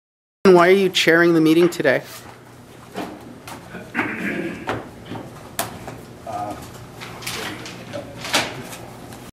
Glen Grant on Why he's chairing in camera council meeting in Cornwall
- Glen Grant on Why he's chairing in camera council meeting in Cornwall - February 19, 2013